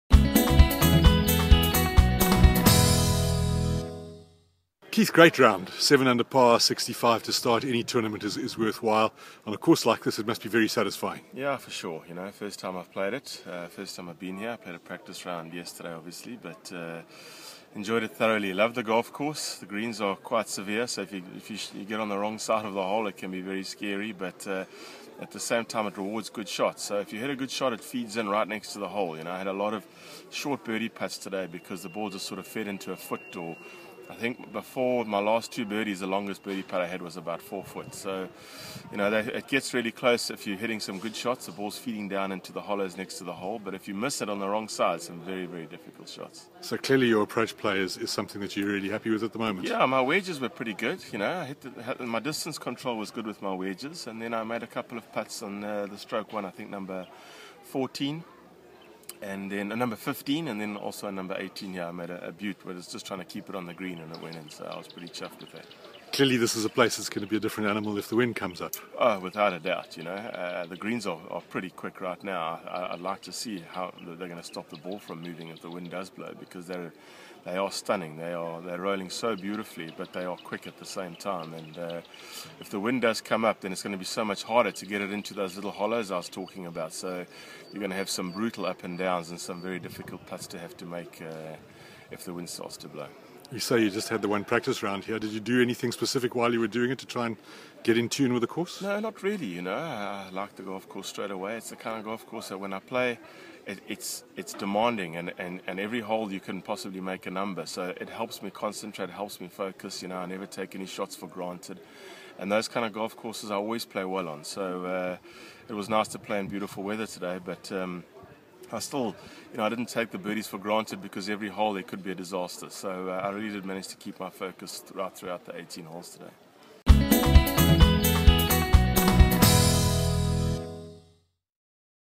Keith Horne round 1
Keith Horne talks about his course-record 65 in the opening round of the Vodacom Origins of Golf presented by Samsung at St Francis Links